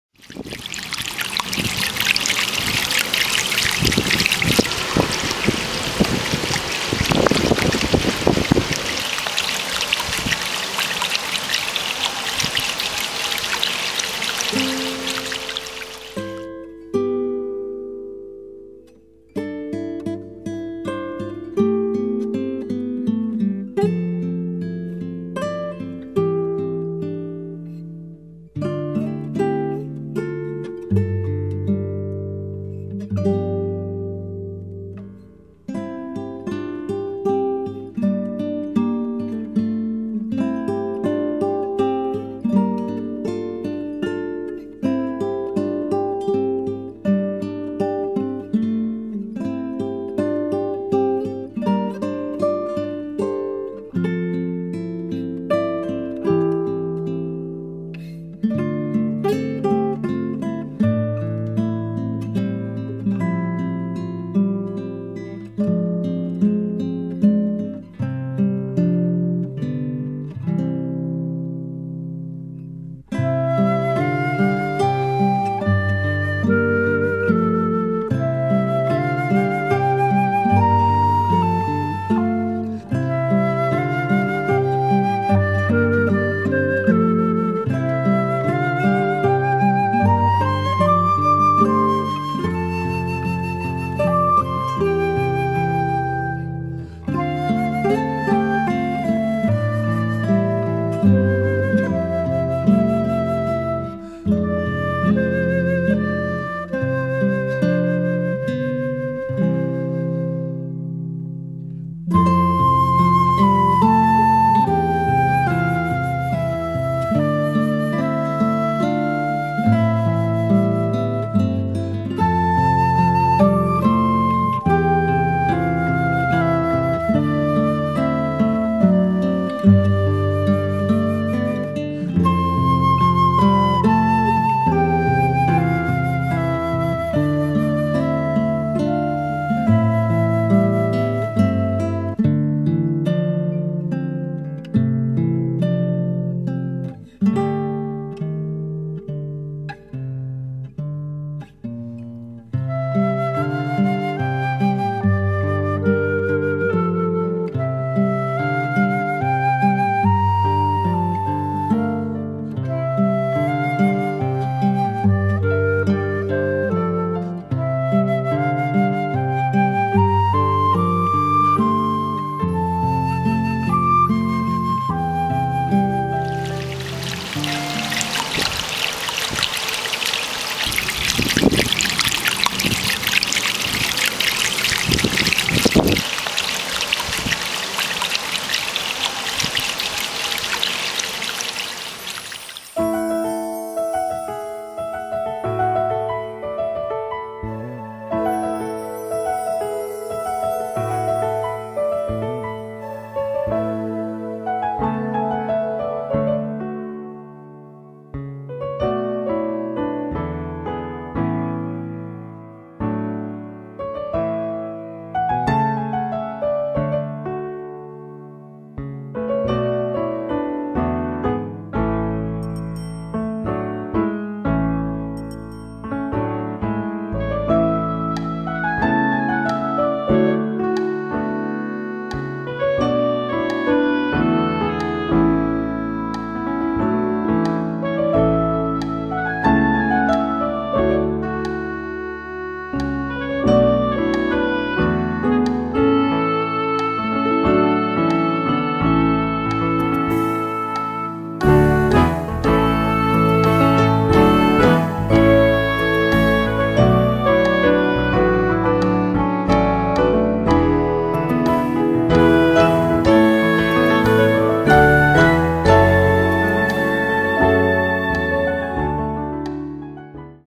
Water of New Life
a presentation of the use of water in Salvation History based on the prayer of blessing used for baptismal water and based on the symbols found in the Baptismal font of Transfiguration